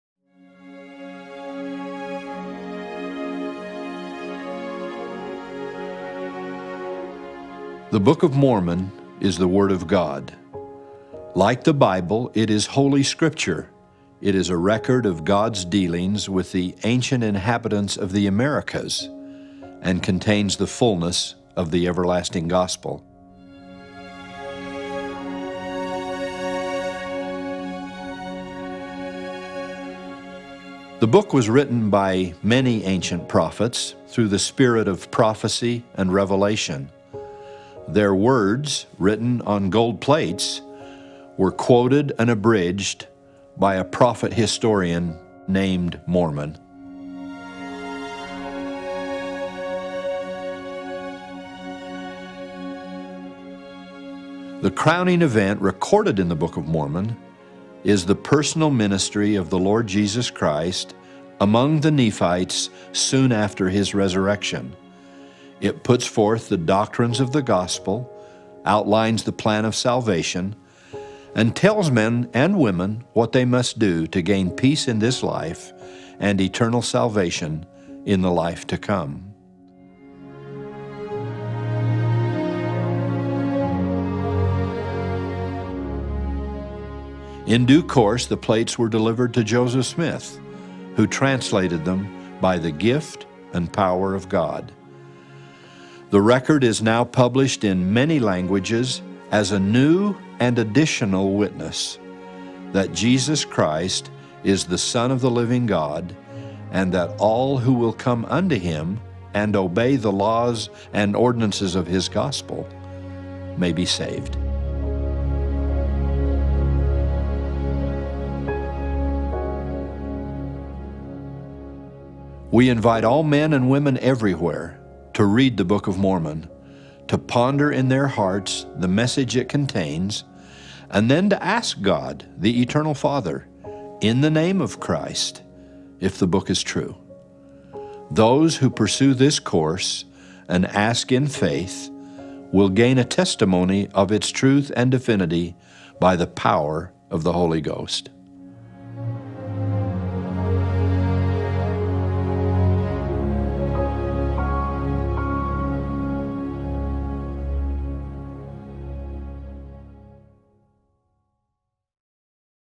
Book of Mormon Introduction | Faith To Act | Come Follow Me 2020
Elder Jeffrey R. Holland explains the background and importance of the Book of Mormon. This book is another testament of Jesus Christ and serves as a companion scripture to the Holy Bible.

(2:42)